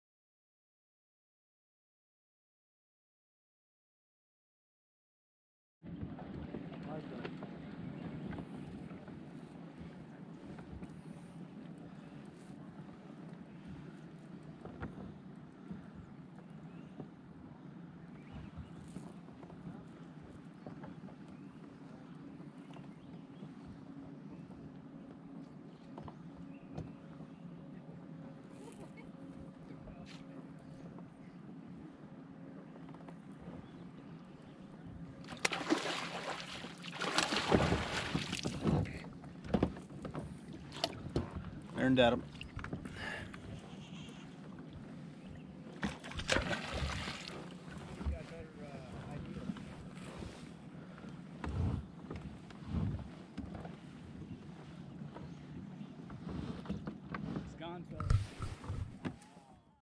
The loss heard around the world 😪. Baker Lake Sockeye 2021. Special 1 year anniversary edition.
7/17/22 marks the one year anniversary of the great "shake-off" of 2021. It also highlights the day I came home with more fish than Mike. Never forget this day ladies and gentlemen. Never Forget! 🤣